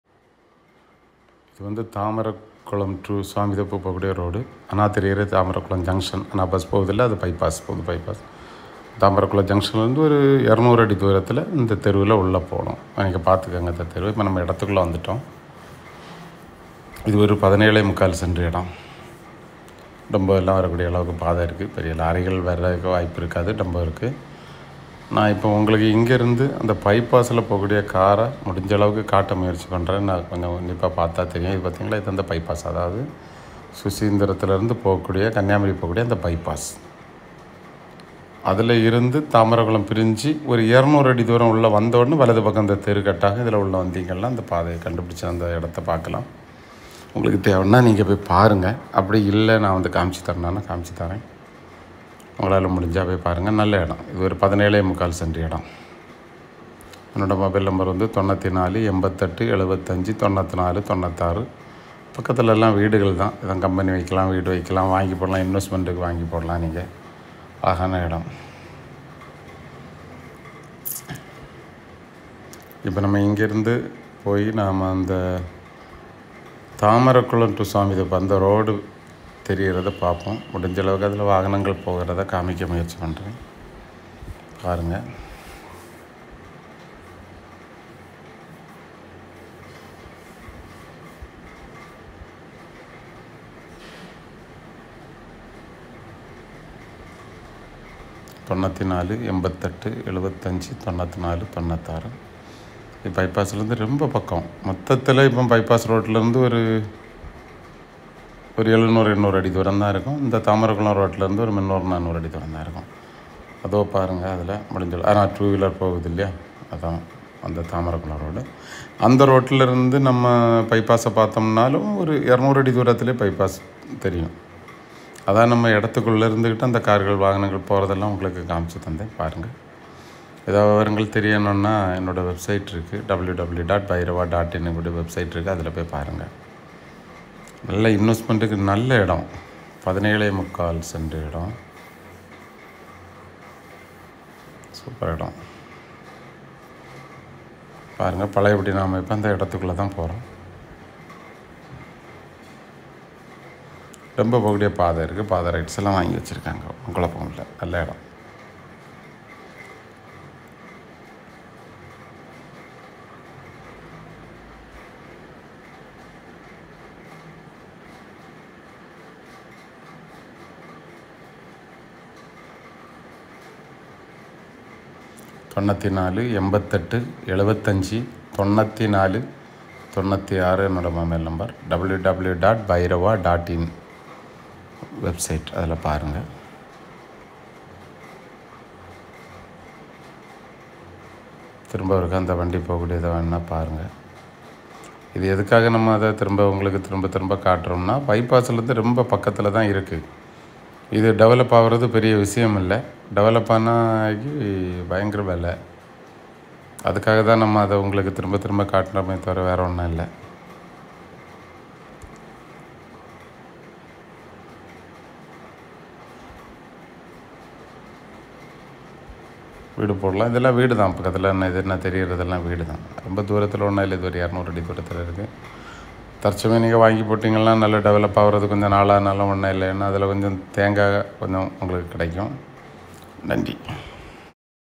17 cent land sale Thamaraikullam,near Susindram,Nagercoil
17 cent land sale Thamaraikullam
20 coconut tree
sub road
600 feet from Thamaraikullam junction
300 feet from Thamaraikullam to Swamy thoppu
600 Susindram to Vazhukkamparai pypass road